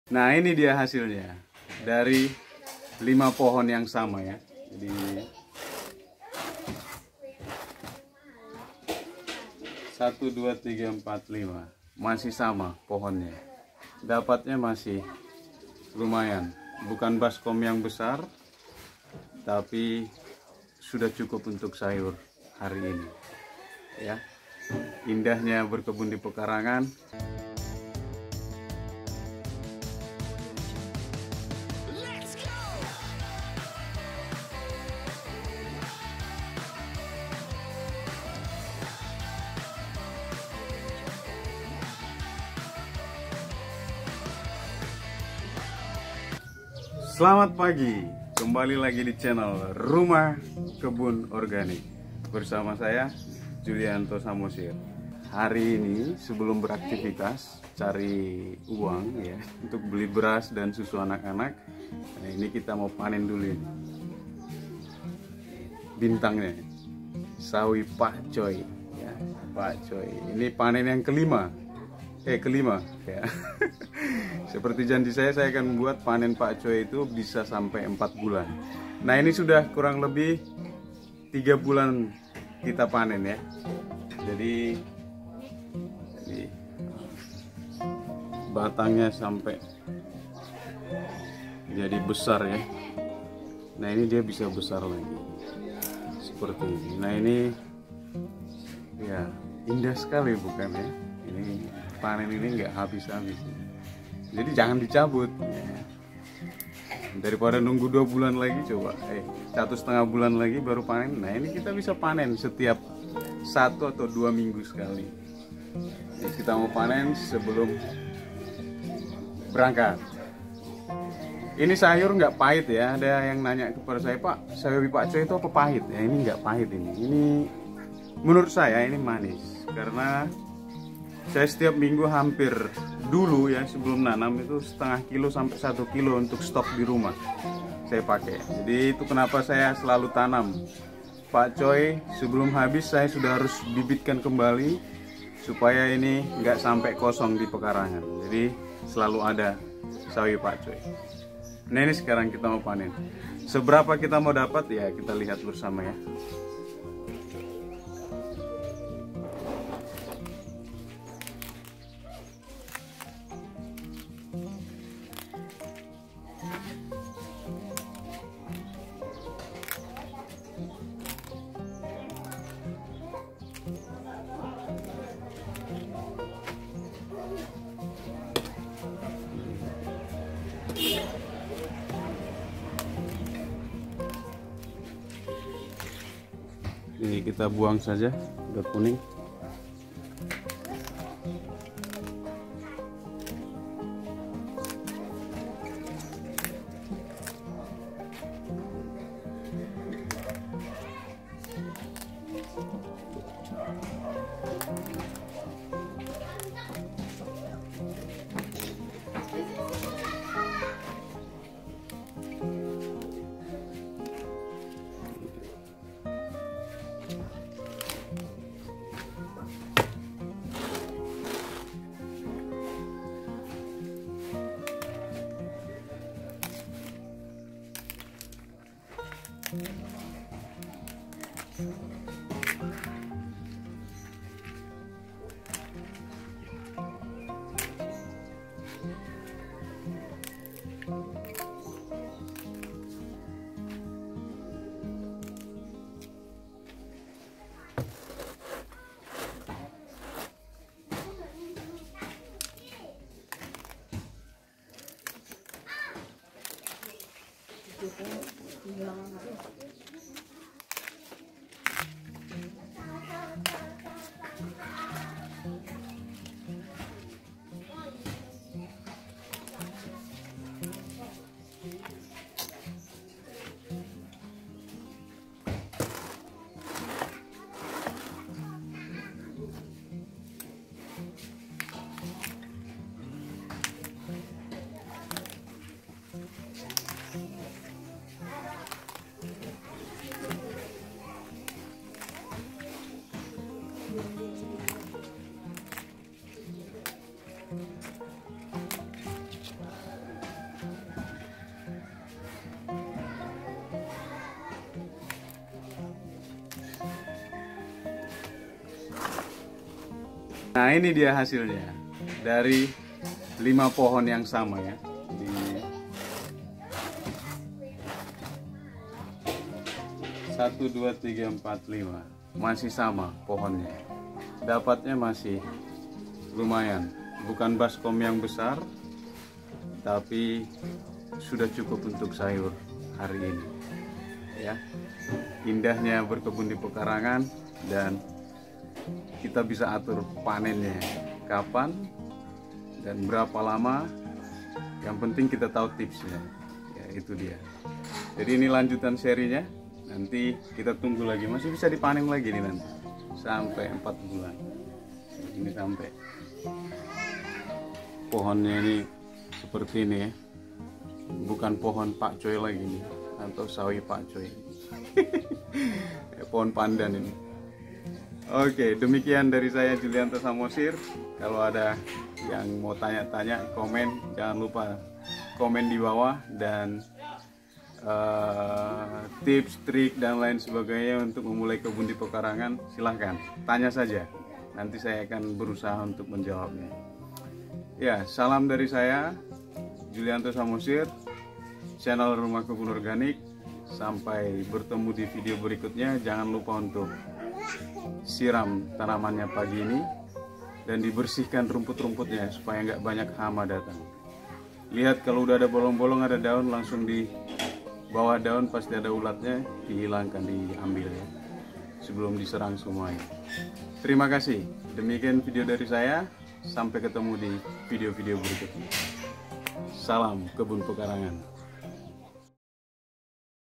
Panen Ke 5 Sawi Pakcoy Dari Pohon Yang Sama, Indahnya Berkebun
Hallo Bapak/Ibu Video ini hanya dokumentasi panen sawi pakchoy yang ke 5 dari pohon yang sama. seperti janji saya bahwa saya akan coba buktikan bahwa saei pakchoy masih bisa tetap bisa dipanen sampai umur 4 bulan asalkan tahu tips panen dan perawatannya. mudah-mudahan bermanfaat.

Salam
Rumah Kebun Organik